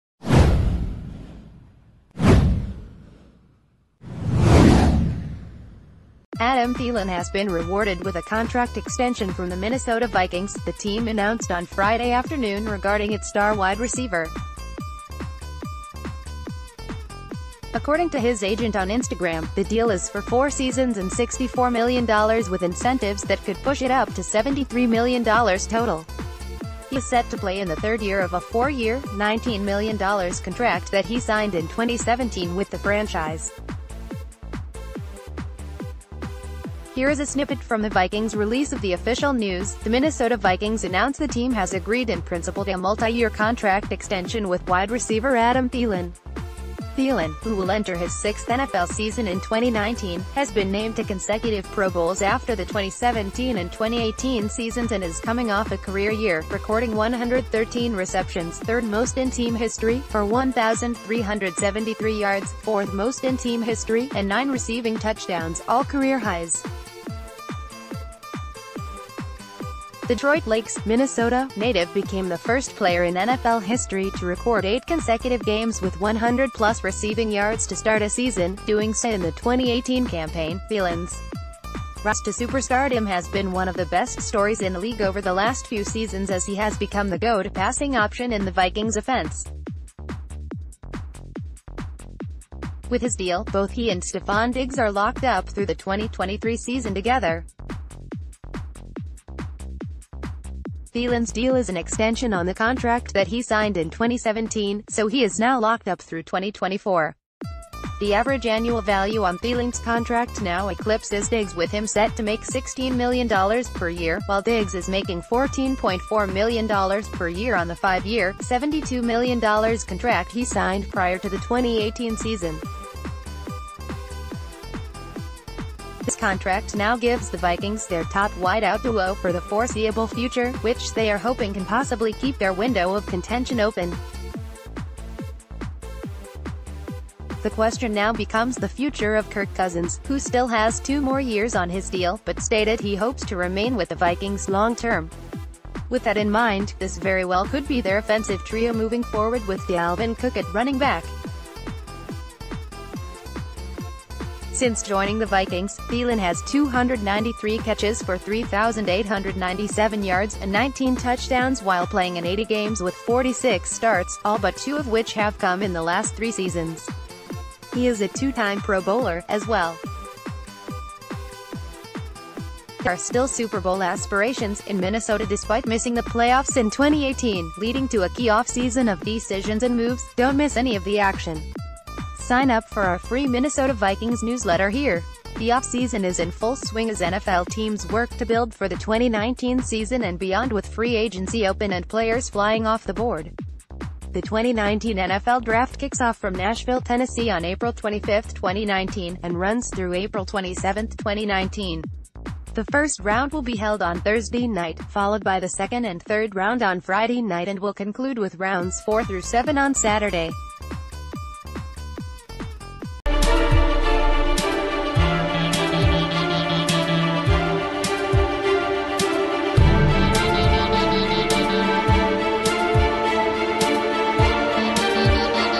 Adam Thielen agrees to contract extension with Minnesota Vikings
Adam Thielen has been rewarded with a contract extension from the Minnesota Vikings, the team announced on Friday afternoon regarding its star wide receiver. According to his agent on Instagram, the deal is for four seasons and $64 million with incentives that could push it up to $73 million total. He was set to play in the third year of a four-year, $19 million contract that he signed in 2017 with the franchise. Here is a snippet from the Vikings' release of the official news: "The Minnesota Vi...